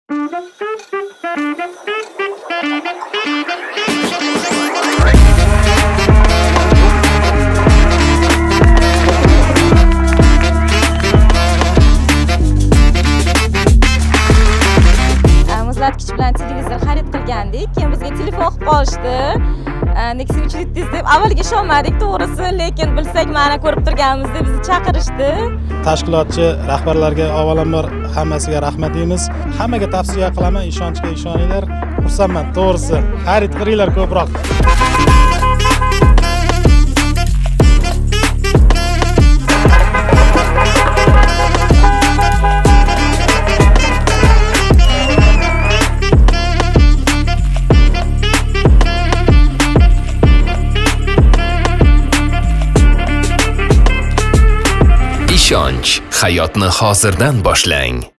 Uchinchi Nexia 3 avtomobili o'z egasiga topshirildi
"Ishonchli mijoz" aksiyasi 3-bosqichi bosh sovrini Nexia 3 avtomobili va boshqa so‘vrinlar o‘z egalariga topshirildi👏
Barcha g‘oliblarimizni chin dildan tabriklaymiz va yutuq muborak bo‘lsin deymiz!

Aksiyamiz davom etmoqda, siz ham ishtirok eting va avtomobil yutib olish imkoniyatini qo‘lga kiriting🤩